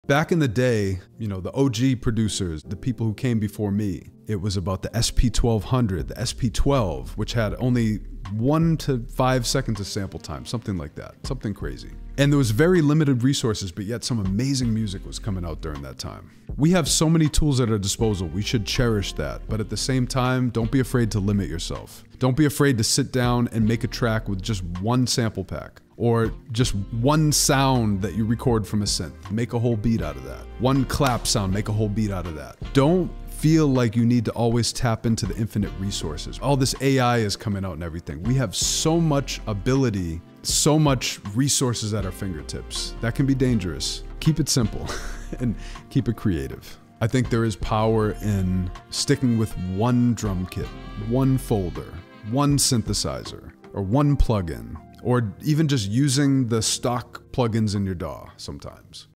Wrapping up my thoughts on having infinite tools at our disposal as music producers 🧰🎶
I wrap up my thoughts on having an infinite amount of tools at our disposal as music producers, and that can be a wonderful thing along with being problematic in the sense of where you should start when making beats.